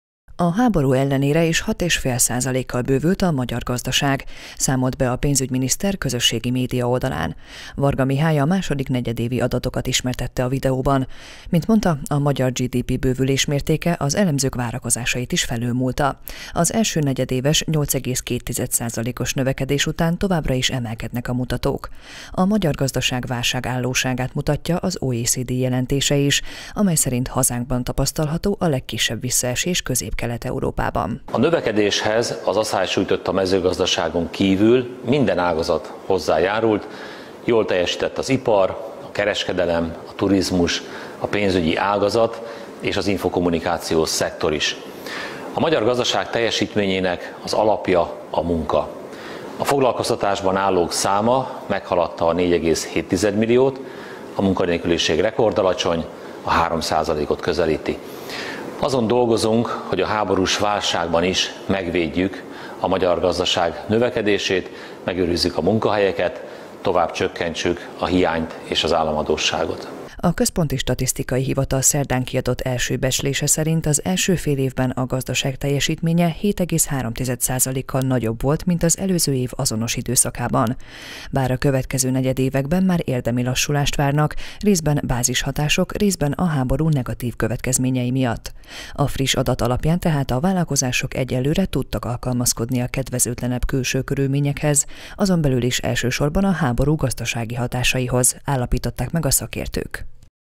A második negyedévben 6,5 százalékkal bővült a hazai GDP
Az ipar, a kereskedelem, a turizmus, valamint a pénzügyi és az infokommunikációs szektorok is hozzájárultak a magyar gazdaság növekedéséhez – mondta a pénzügyminiszter. Varga Mihály bejelentésében kiemelte, hogy a második negyedévben 6,5 százalékkal bővült a hazai GDP. Úgy fogalmazott: a foglalkoztatottak száma meghaladta a 4,7 milliót, míg a munkanélküliségi ráta a 3 százalékhoz közelít.